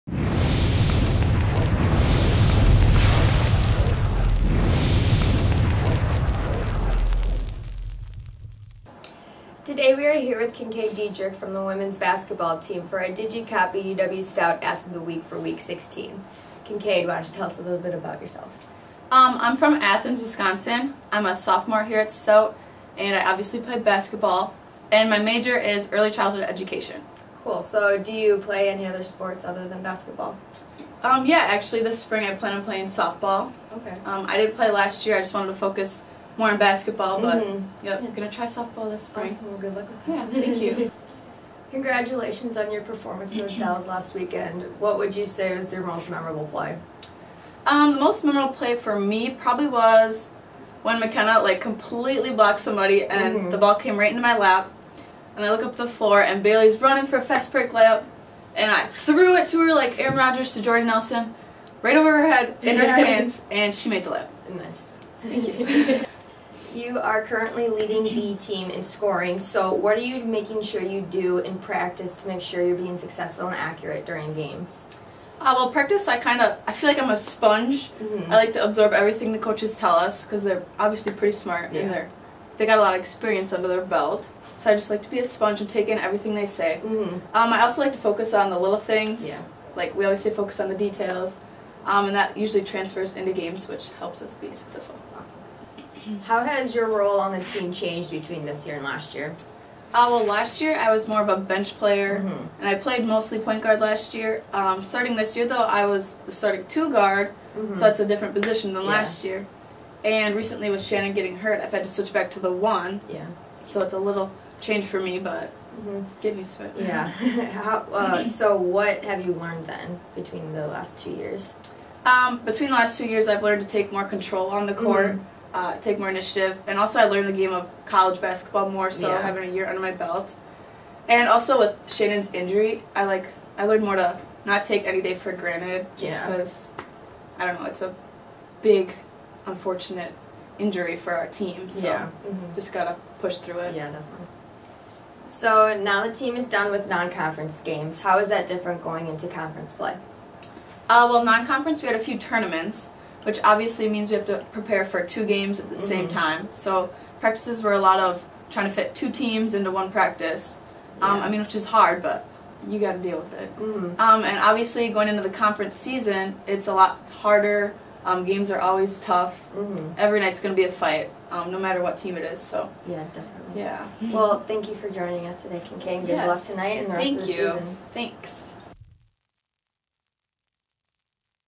Kyncaide Diedrich, DigiCopy UW-Stout Athlete of the Week #16 (16-17)
Kyncaide Diedrich wins the DigiCopy UW-Stout athlete of the week honors after leading the team in scoring both games last week and tying a career best 22 points in one of those games.